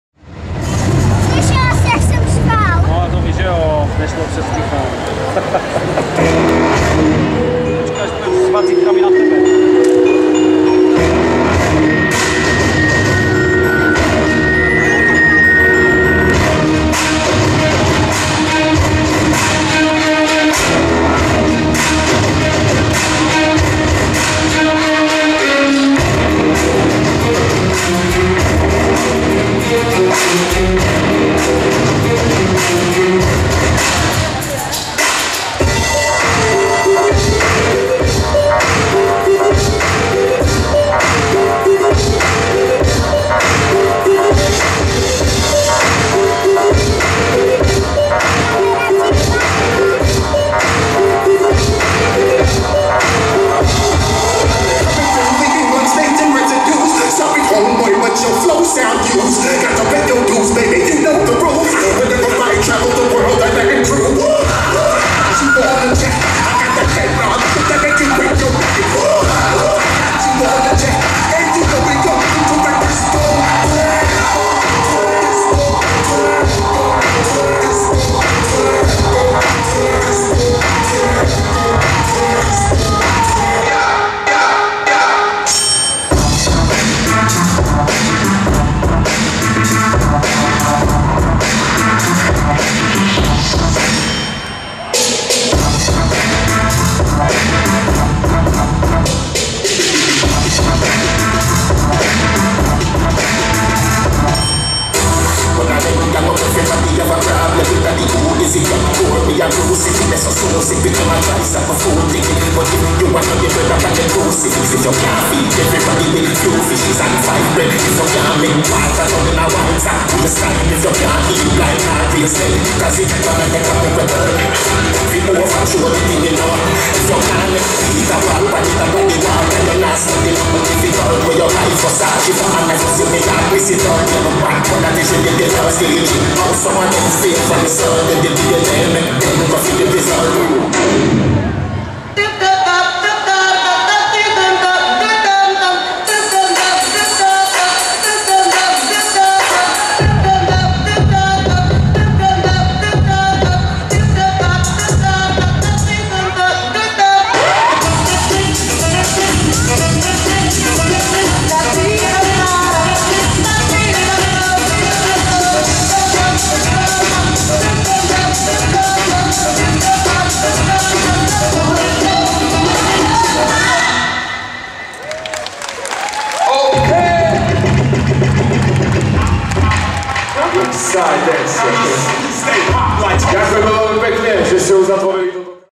Xside DANCERS - New challenge (IDO hip hop junioři)
Mistrovství České republiky - Beat Street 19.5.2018
2. místo